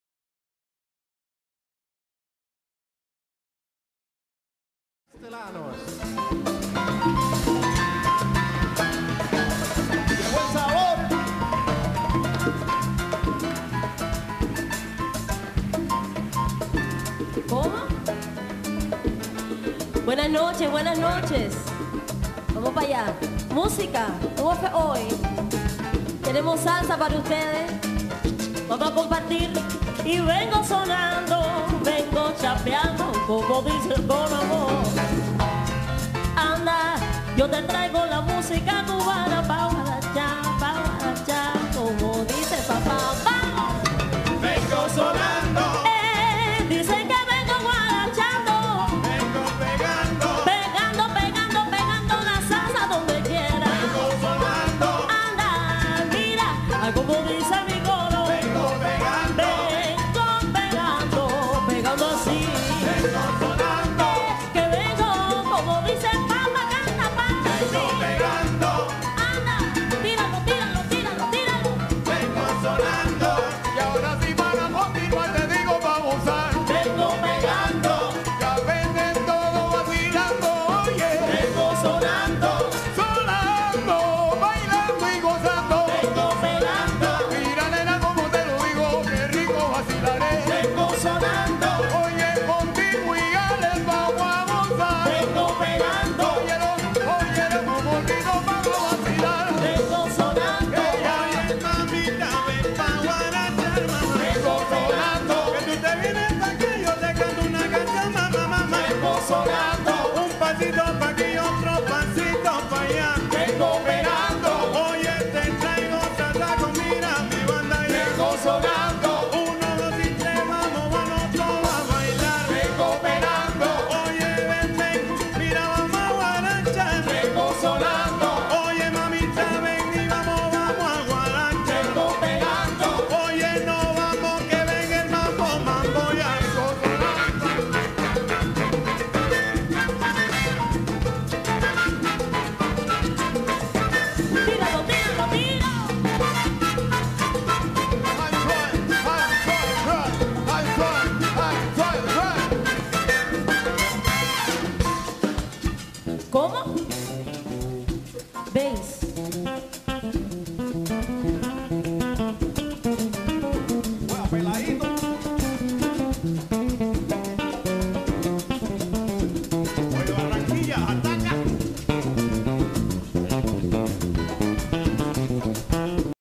SOL NACIENTE in COncert 20.7.2007 featuring Olvido
SOL NACIENTE in Concert featuring Olvido Ruiz CAstellanos, Oslen Ceballo Brian , Lennart Axellson and the Band. Concert from the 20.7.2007 in Lüneburg, directed by DFK Brasil